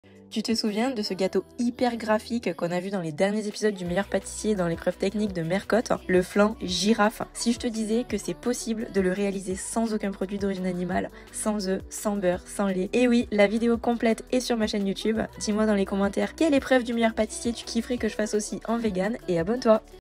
FLAN GIRAFE 🦒 (épreuve technique de Mercotte dans le Meilleur Pâtissier saison 12)
🦒 Le flan girafe de Mercotte en version vegan 🦒
Tu te souviens de ce gâteau hyper graphique qu’on a vu dans les derniers épisodes du Meilleur Pâtissier, dans l’épreuve technique de Mercotte et Titouan Claudet ? Le flan girafe ! 🦒
Si je te disais que c’est possible de le réaliser sans aucun produit d’origine animale ? Sans oeufs, sans beurre, sans lait ! Et oui, la vidéo complète est sur ma chaine Youtube !
Dis-moi dans les commentaires quelle épreuve du Meilleur Pâtissier tu kifferais que je fasse aussi en vegan.